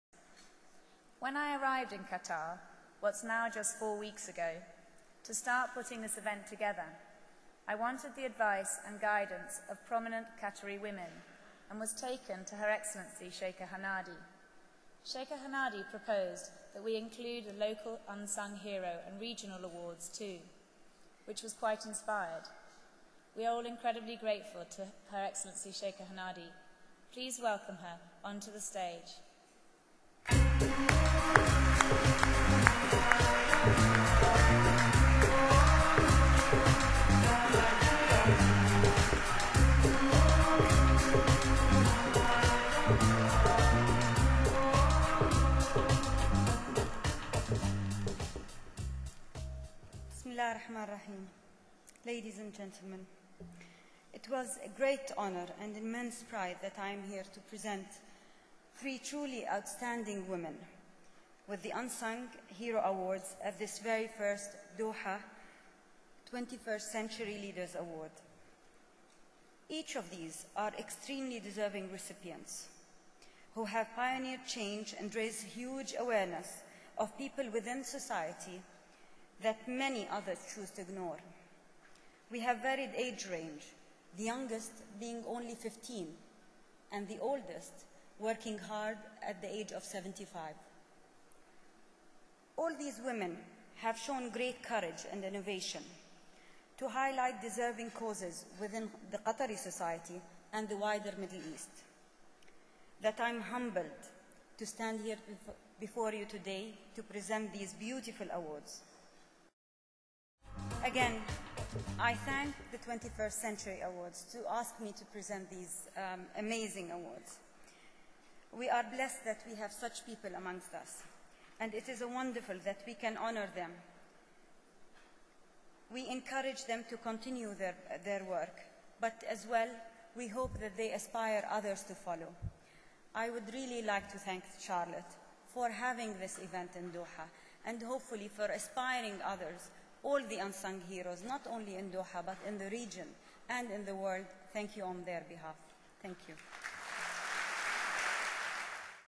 HH Sheikha Hanadi Bint Nasser Bin Khaled Al Thani kind host at the 21st Century Leaders Awards
At the awards HH Sheikha Hanadi Bint Nasser Bin Khaled Al Thani kindly awarded three outstanding women, two from Qatar and one from Palestine with the first 21st Century Leaders Unsung Hero Awards.